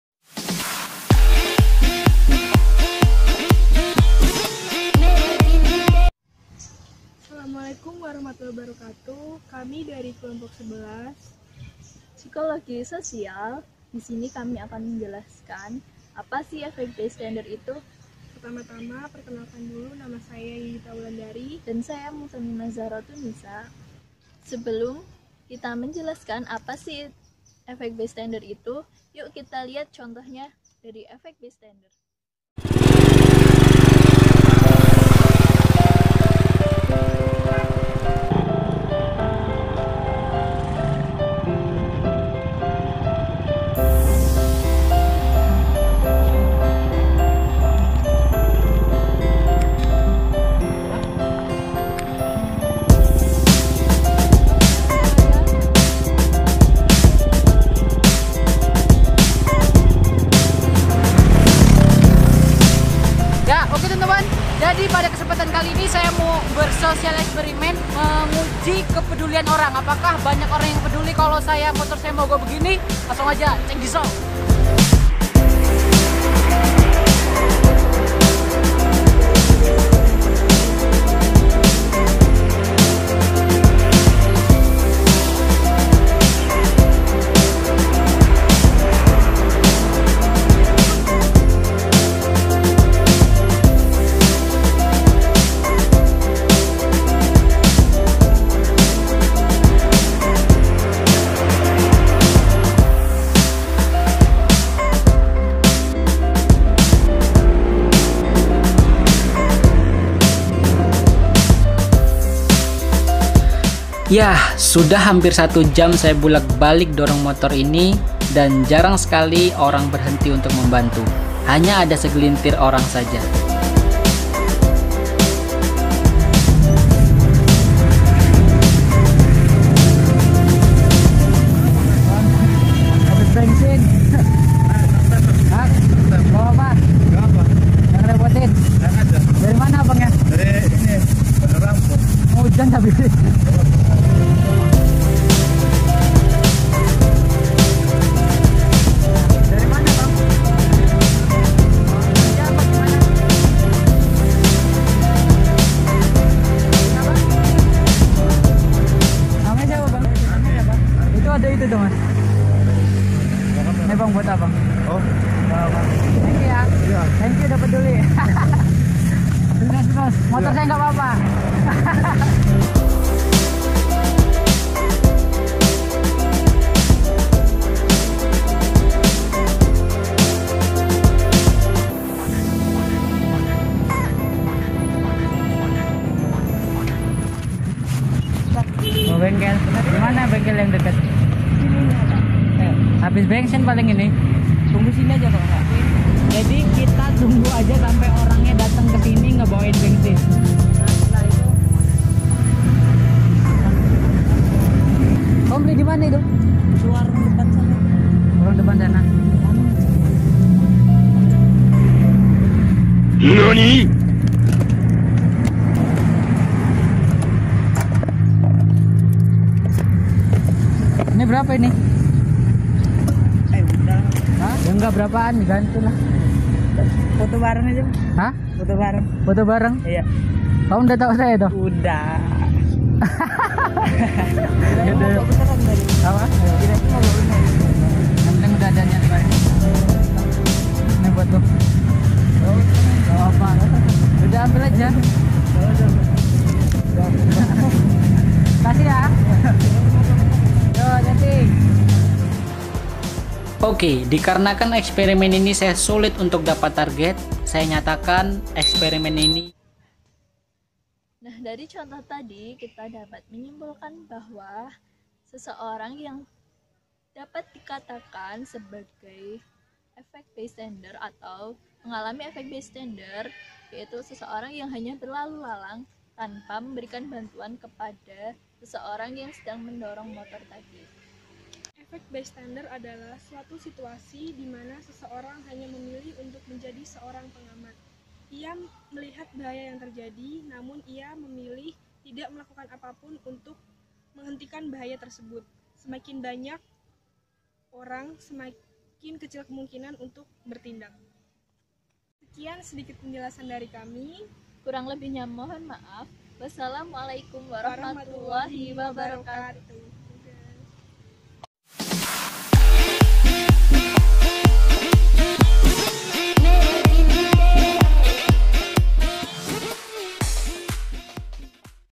Bystander: Penjelasan dan Contoh - Vlog Psikologi Sosial - Rb.4
Tugas vlog psikologi sosial karya mahasiswa Psikologi FIP UNNES